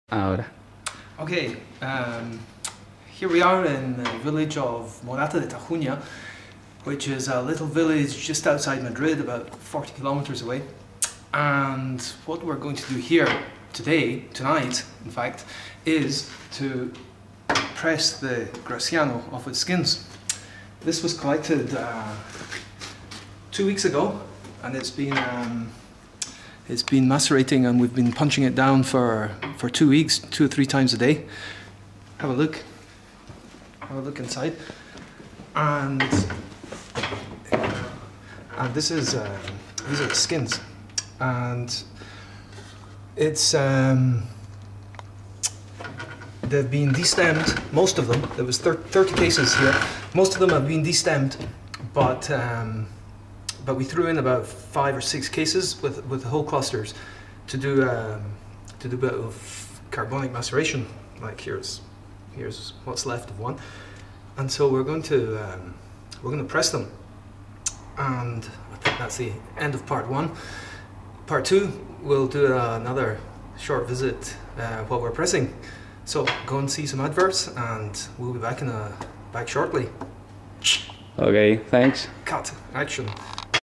Pressing Graciano - Part A.mpg
Pressing Graciano grapes by hand in Morata de Tajuña (Madrid, Spain). 2011